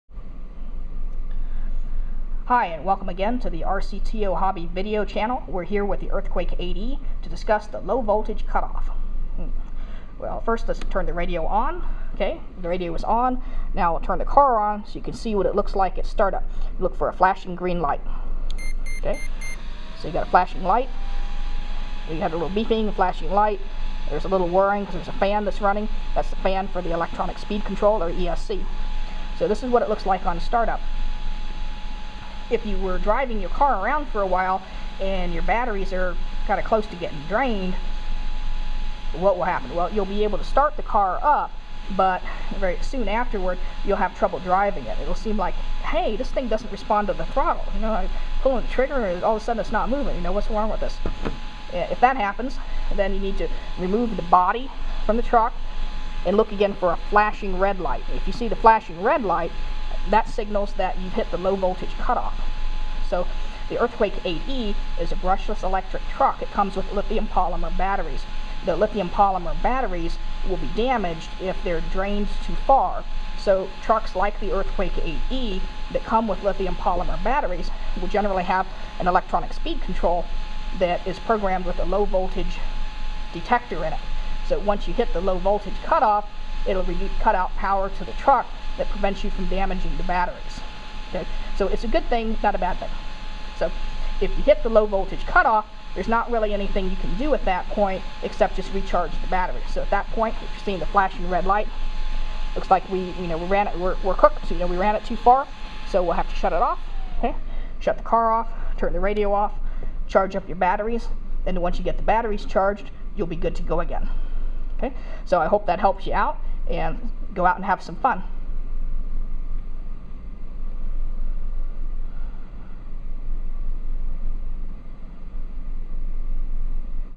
ESC Low Voltage Cutoff - Redcat Racing Earthquake 8E
What is the ESC low voltage cutoff, or LVC, in a brushless rc truck and what does it do?  LiPo batteries cannot be drained too far without causing them damage.  The electric speed control, or ESC, on most brushless electric trucks detects when the battery voltage is getting too low and cuts power to the motor to avoid damaging the batteries.  We discuss the LVC for the Redcat Racing Earthquake 8E, but this applies to other rc cars and trucks as well.  What's the solution when you hit the limit?  Charge the batteries, of course!